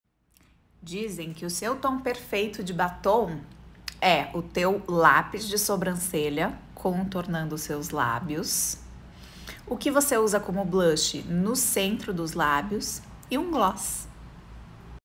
TOM PERFEITO DE BATOM
Você sabe como chegar no seu tom perfeito de batom?
Use a maquiagem como uma aliada para realçar a sua beleza natural.
Realce sua beleza natural.